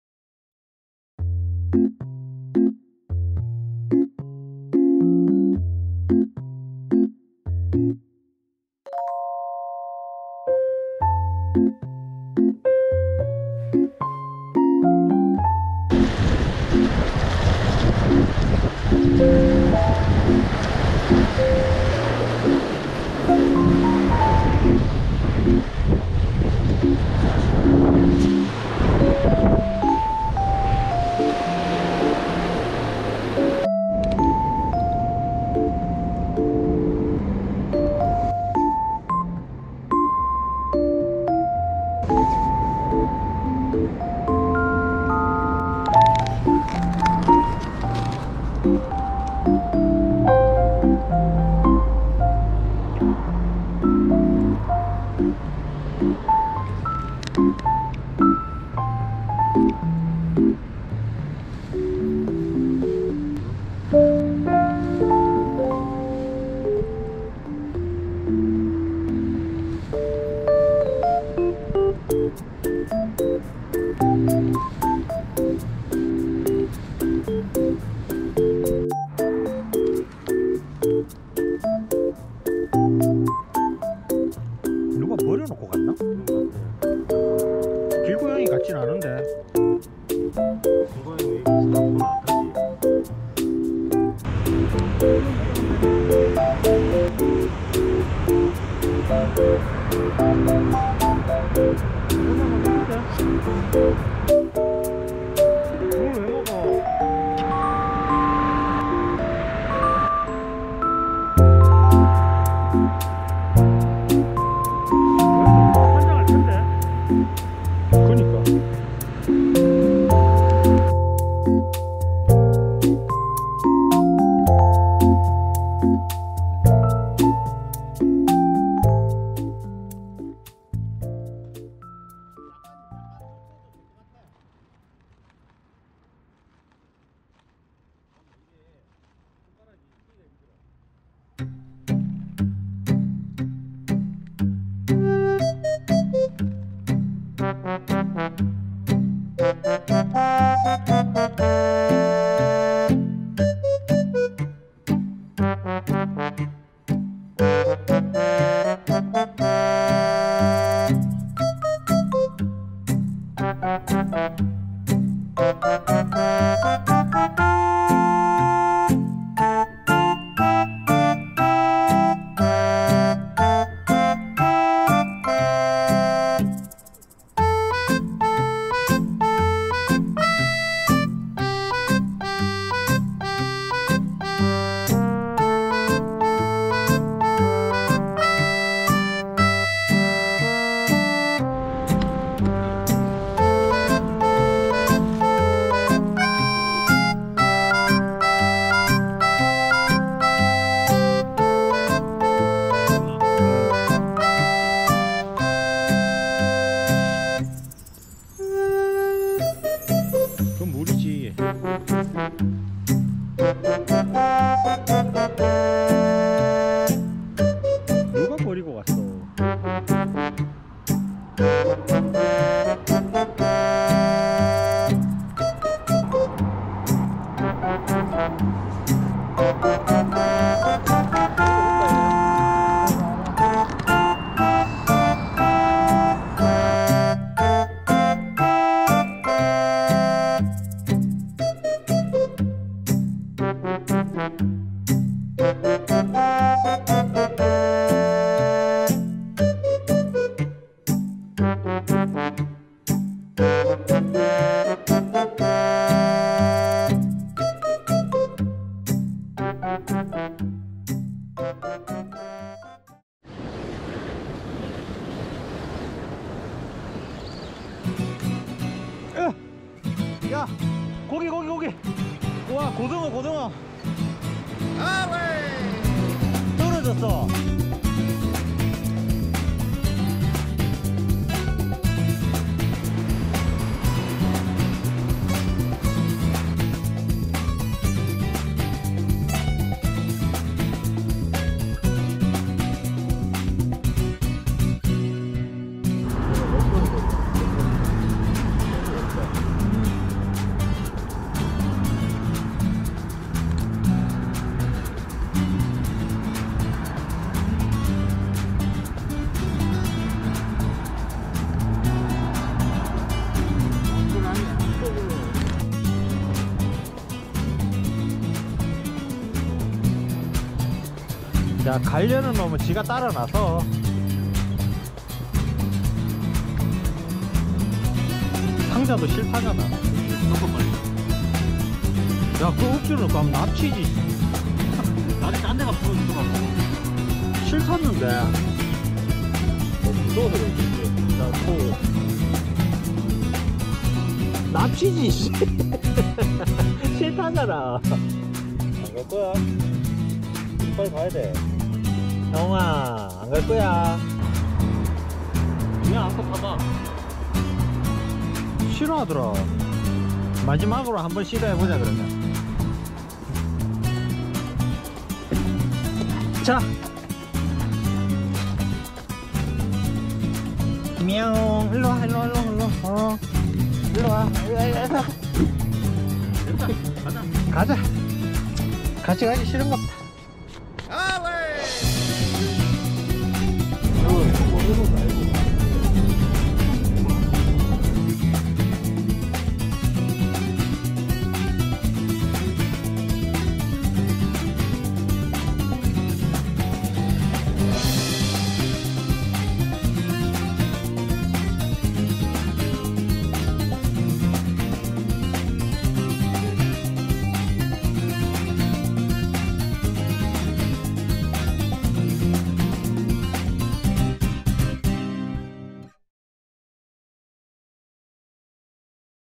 낚시 갔다가 불쌍하게 웅크리고 있는 고양이를 만났습니다. 냥줍? 힐링영상
노숙냥 ㅠ.ㅜ